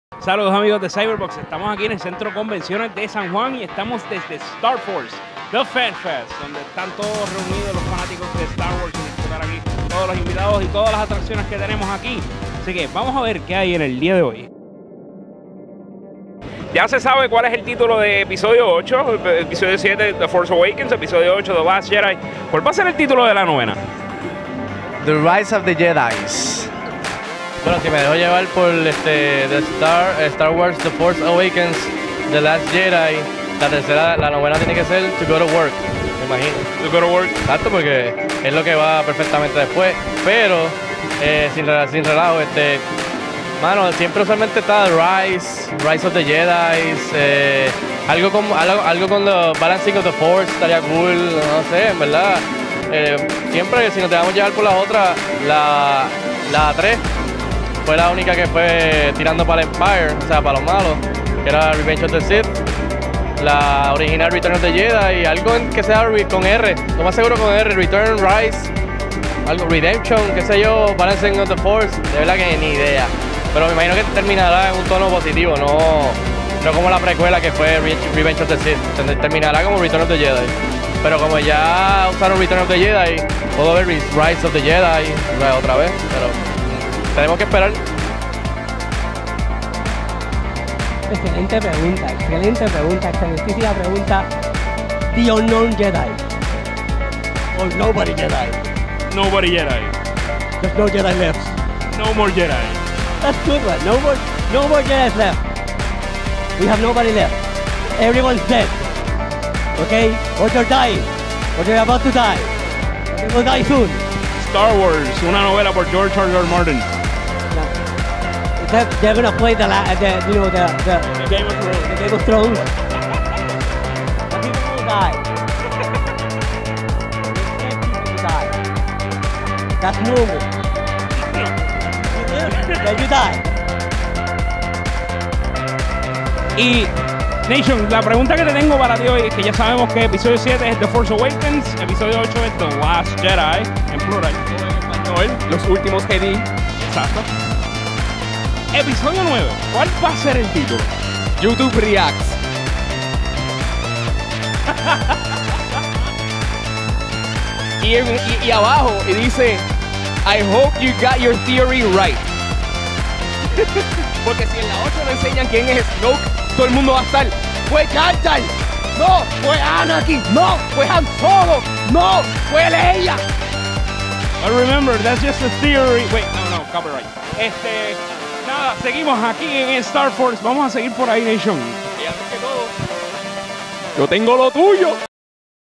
¿Cual debería ser el titulo del noveno episodio de Star Wars?
Ángel estuvo en Star Force preguntándole a varias personas cual debería ser el titulo del noveno episodio de Star Wars.